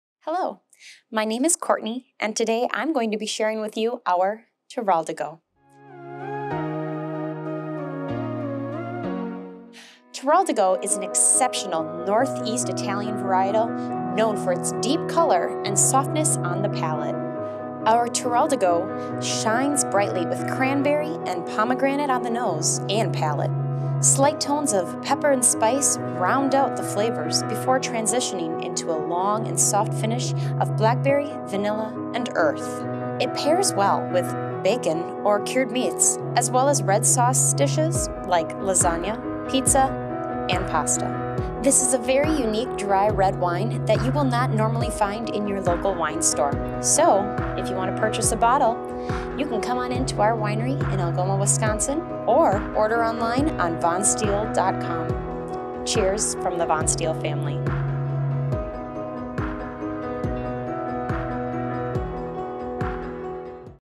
Virtual Tasting: Teroldego
Our assistant winery manager Courtney talks through our new Teroldego dry red wine!